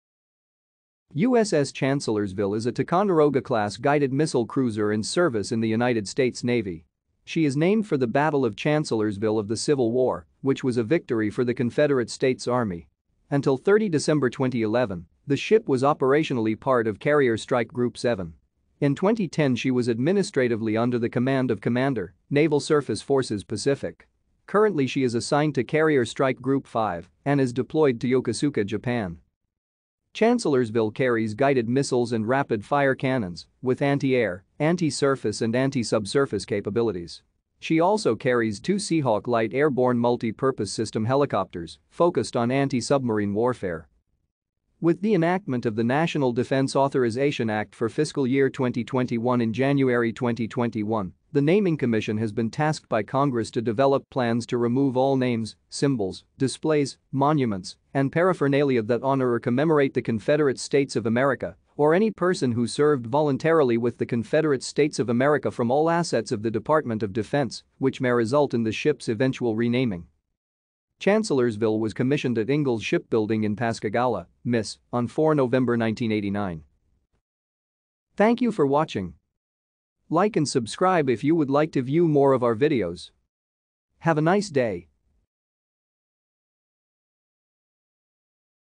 USS Chancellorsville
USS Chancellorsville  is a Ticonderoga-class guided-missile cruiser in service in the United States Navy. She is named for the Battle of Chancellorsville of the Civil War, which was a victory for the Confederate States Army. Until 30 December 2011, the ship was operationally part of Carrier Strike Group Seven. In 2010 she was administratively under the command of Commander, Naval Surface Forces Pacific. Currently she is assigned to Carrier Strike Group Five and is deployed to Yokosuka, Japan.